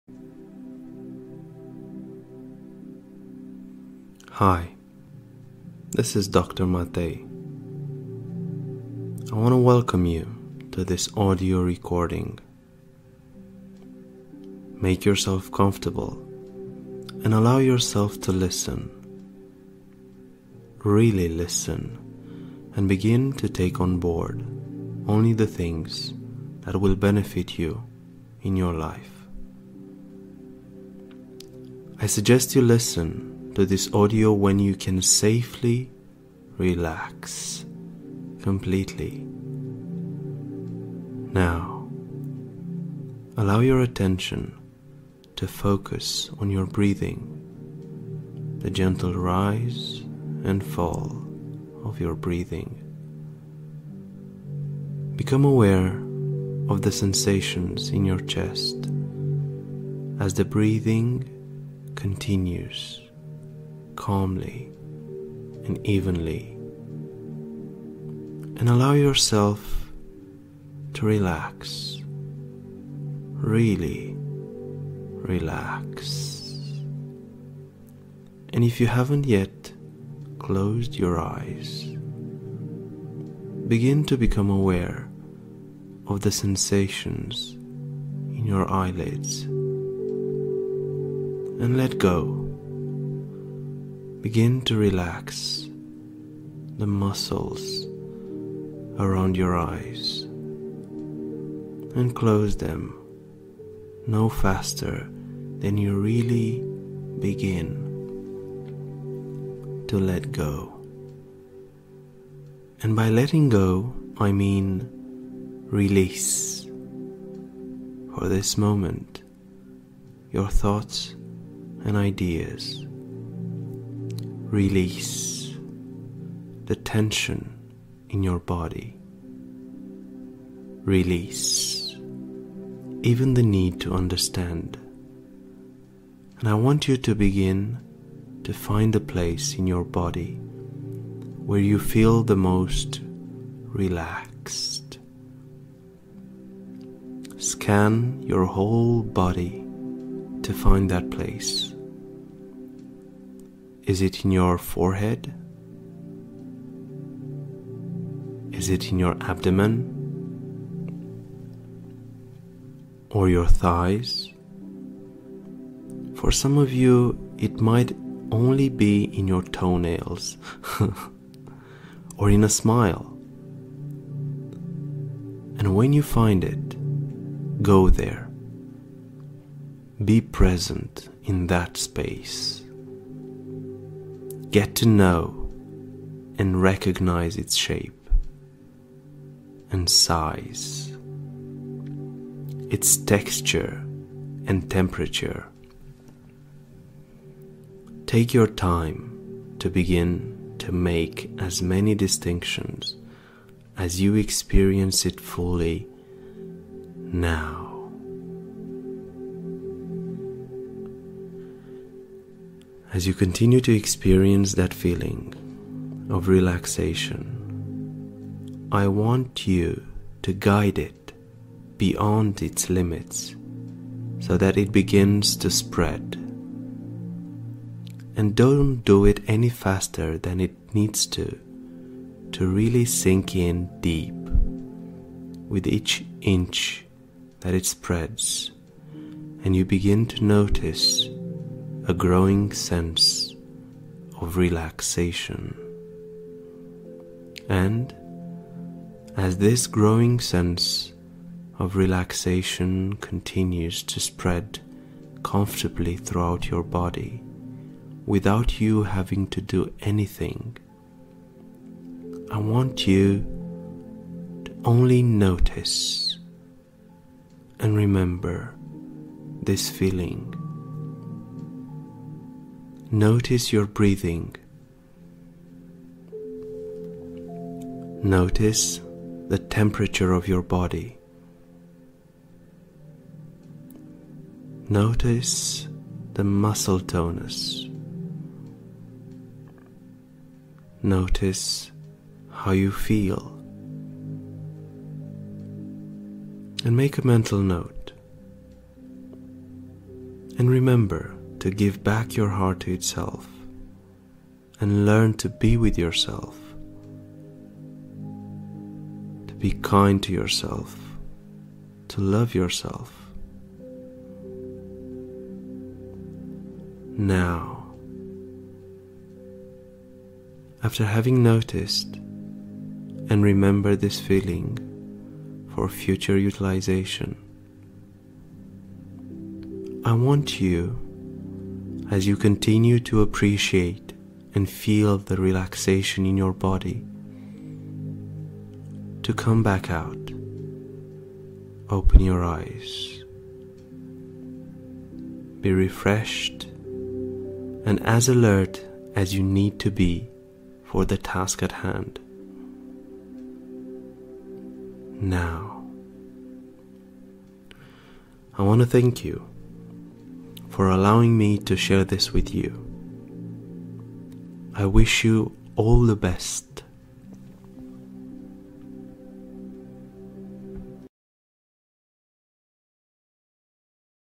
Connect with yourself
I want to welcome you to this audio recording. Make yourself comfortable and allow yourself to listen... really listen and begin to take on board only the things that will benefit you in your life.
I suggest you listen to this audio when you can safely relax completely!